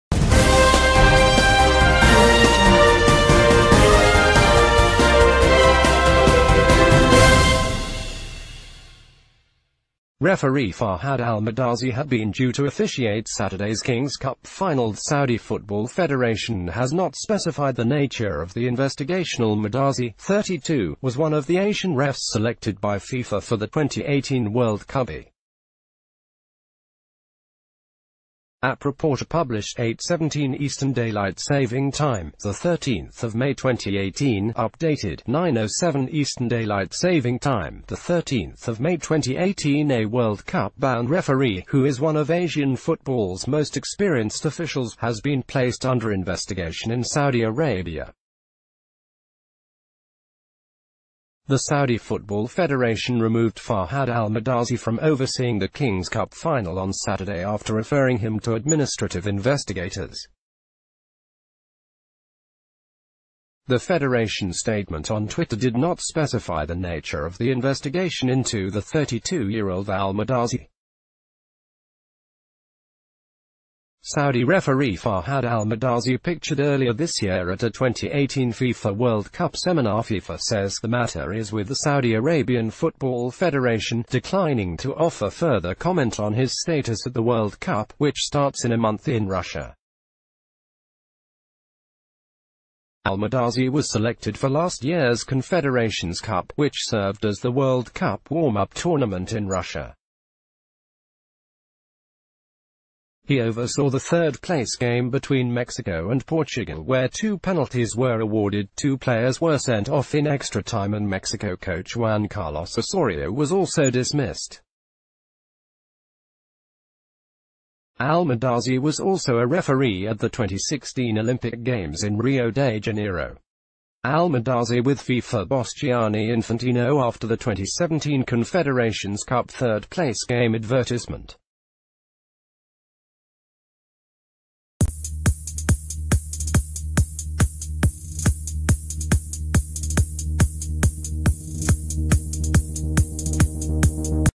Breaking News 24H -World Cup referee Fahad Al Mirdasi is being investigated in Saudi Arabia
Referee Fahad Al Mirdasi had been due to officiate Saturday's King's Cup finalThe Saudi Football Fed...